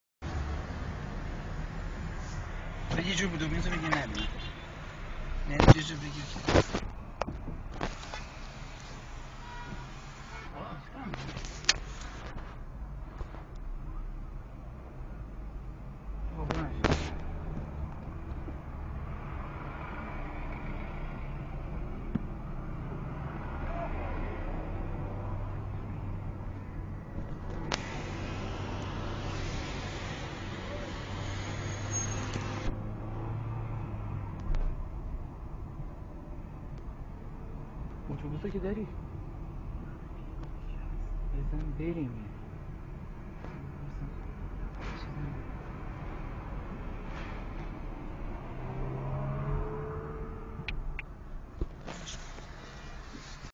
Student protest at Medical Sciences University Iran 31 Oct 2010
امروز دانشگاه علوم پزشکی ایران یک دفعه به دستور وزیر بهداشت منحل اعلام شد. دانشجویان و اساتید که با شنیدن خبر انحلال دانشگاه شوکه شده بودند تحصن کردند. دولت کودتا برای کنترل اوضاع، پلیس ضد شورش به اطراف دانشگاه فرستاده .. شرم آور است Sunday October 31st 2010 : The dissolution of Iran's University of ...